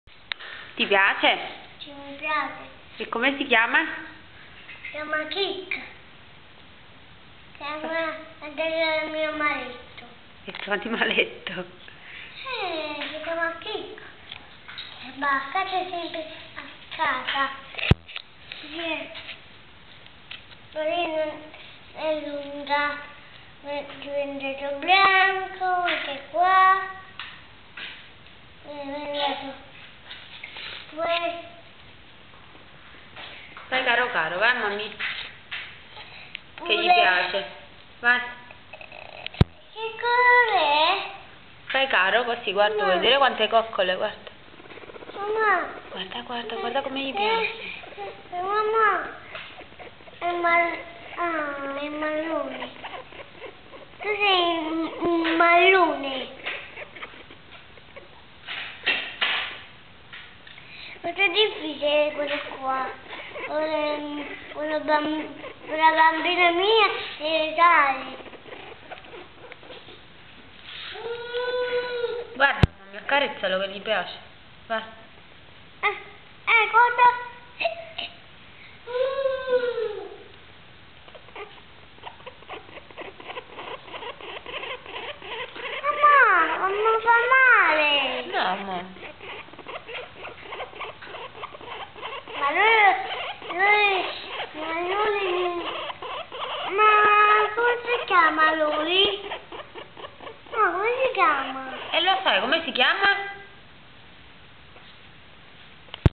Fatina83
Crik in preda alle coccole del piccolo Gennaro!!!